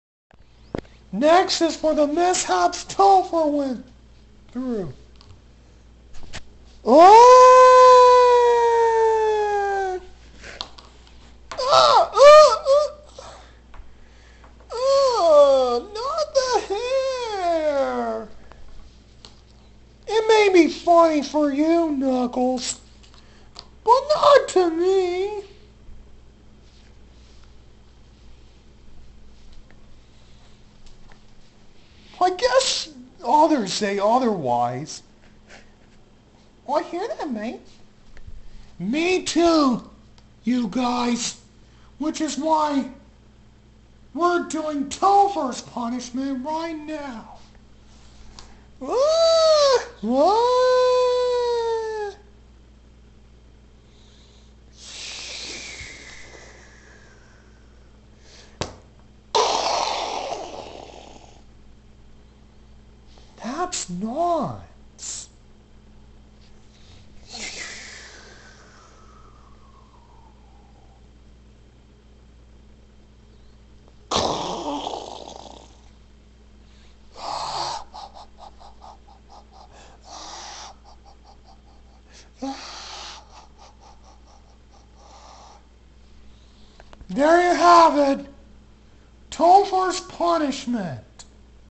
The finale of foolishness Part 3
The third part is Topher's punishment.

The cast:
Nick as Terrafin
Achmed as Eruptor
Peanut as Boomer
Topher as Blades
Knuckles as Drill Sergeant

The rest of my collection so far is the audience.